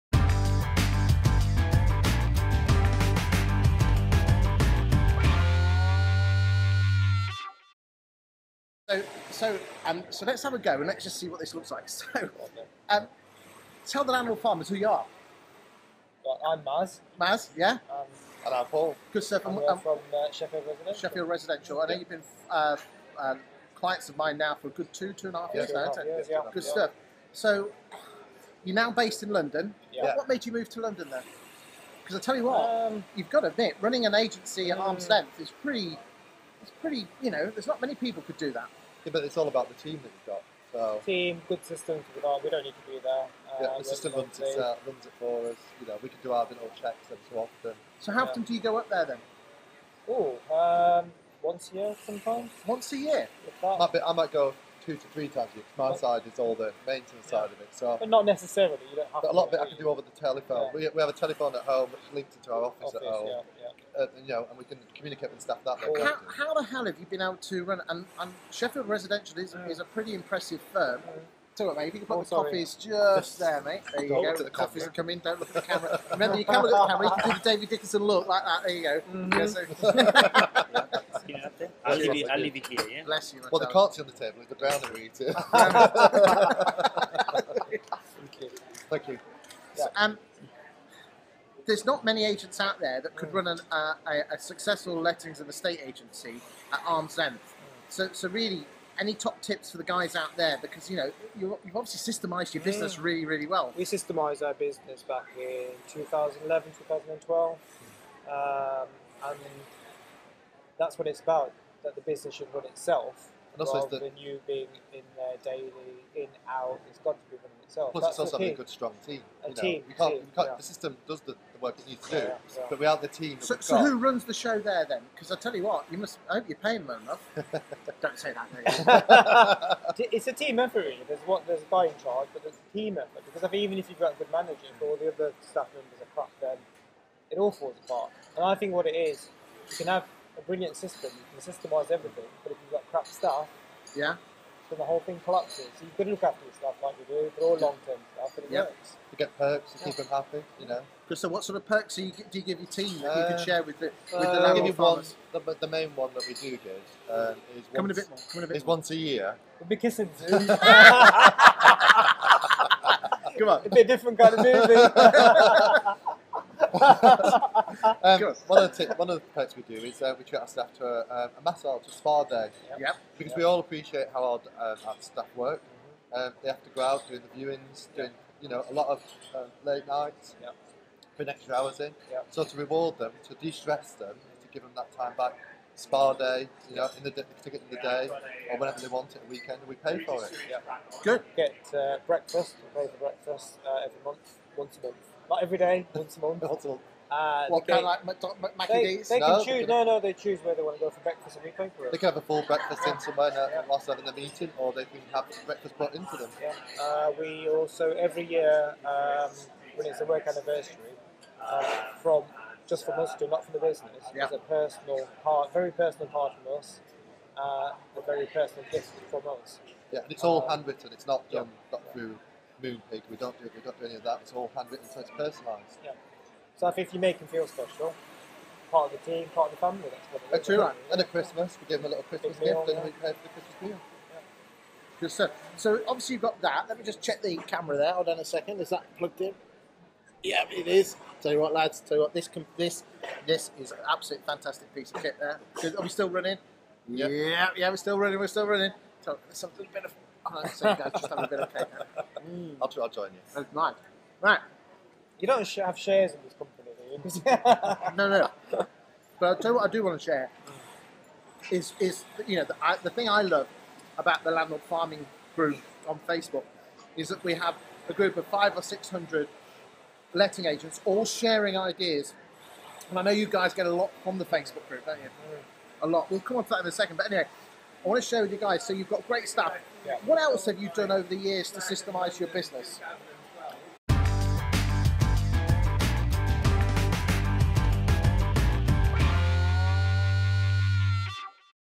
11 Tips That Will Make You an Awesome Letting Agent (Part 1 of 3)
In this 5 min (ish) video, Maz Adam Iqbal and Paul Chambers, who own a couple of successful Letting and Estate Agencies in Sheffield share their top 11 tips on how they can operate their agencies at arms length from London; whilst retaining good staff, keeping costs moderate, having two agencies over 100 miles away from them that they visit only a couple of times a year …and done so successfully for years.

The videos are set into 3 parts

Over the three videos, they share their 11 tips which enable them to run them successfully, and those tips include ..

1. Getting Staff involved in all the Business Decisions
2. Working on the Business instead of in the business
3. Not taking any c**p personally
4. Walking away from cheep fee landlords
5. Having great staff and colleagues
6. Having great systems
7. Controlling costs
8. Going Paperless
9. Customer Care
10. Marketing that works
11. Training the staff